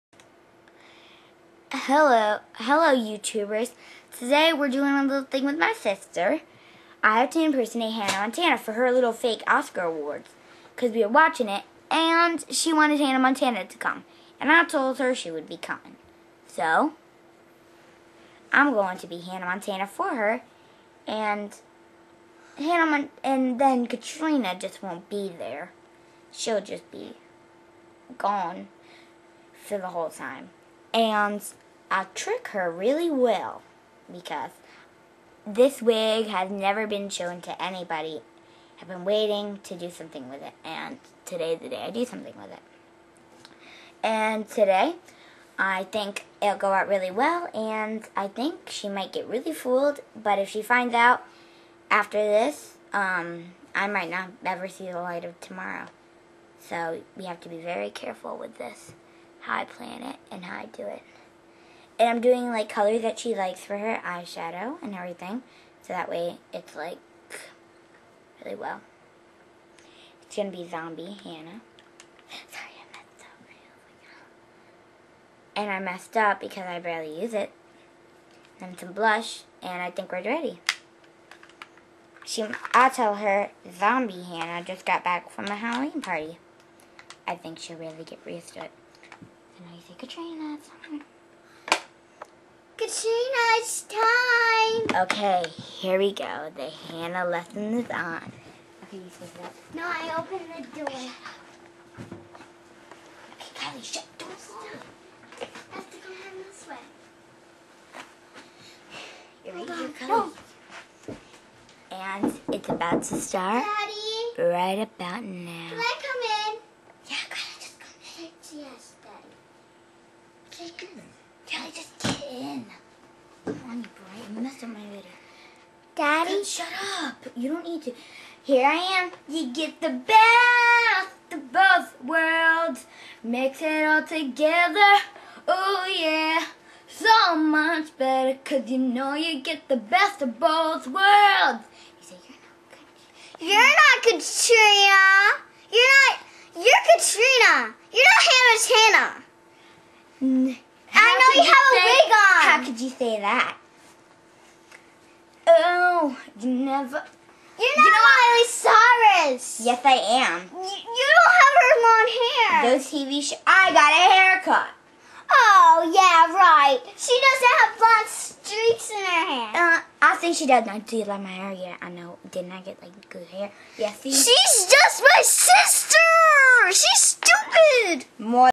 Katrina As Hannah Montana ( Miley Cyrus )
Katrina thought it would be fun to put on ALOT of makeup and be Hannah Montana for Kylee